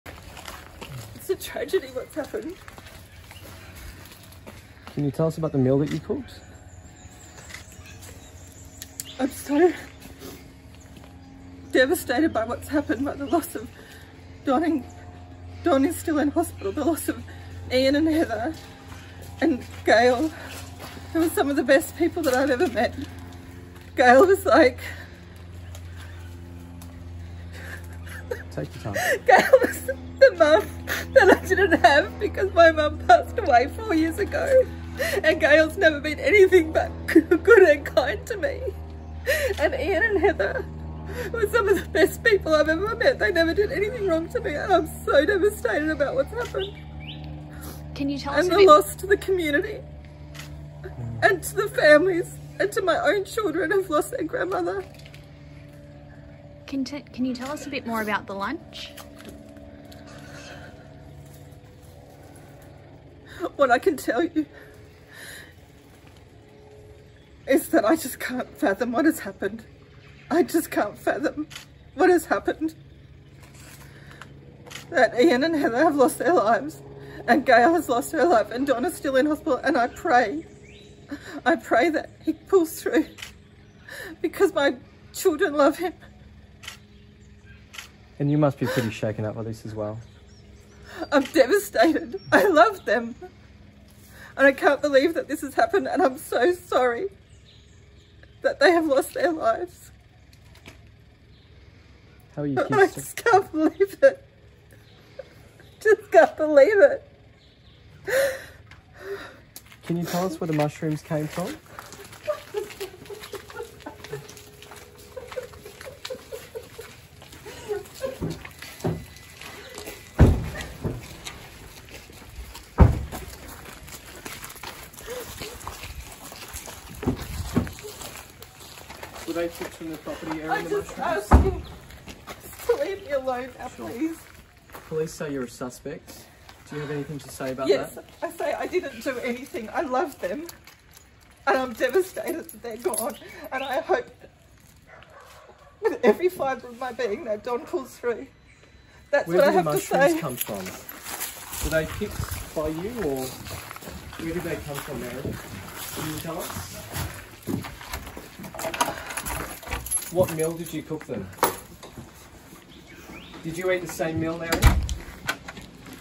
Mushroom poisoning mother speaks out
An emotional Erin Patterson has broken her silence after three people died from eating poisonous mushrooms at her Leongatha home. The dead or sick were fed what police believe were death cap mushrooms on July 29. Death cap mushrooms are so toxic they can cause organ failure. Police are now carrying out forensic testing on a dehydrator that was found at a nearby tip.

Erin Patterson and her two children, who were fed a different meal, did not fall critically ill.